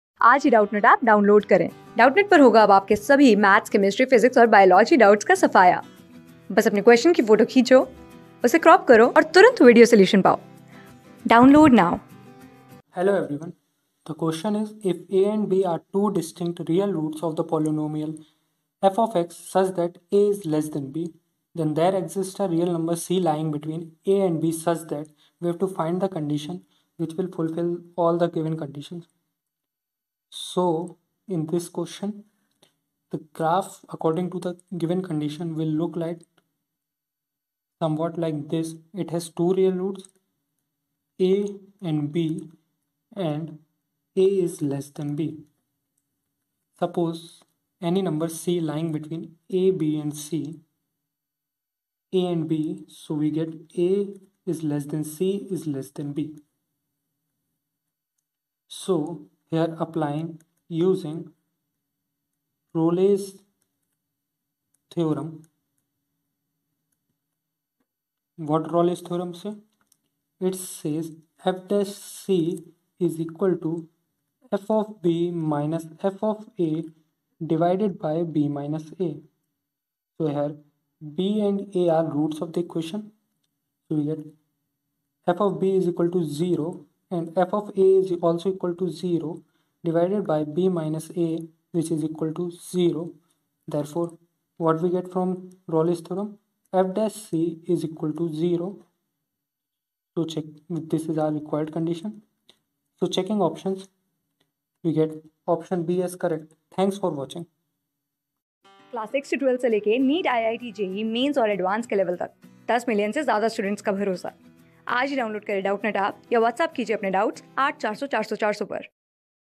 Ifa and b are two distinct real roots of the polynomial f(x) such that a lt b, then there exists...
Ifa and b are two distinct real roots of the polynomial f(x) such that a lt b, then there exists a real number c lying betweena and b, such that
Class: 11
Subject: MATHS
Chapter: QUADRATIC EXPRESSIONS AND EQUATIONS
Board:IIT JEE

👉 Have Any Query? Ask Us.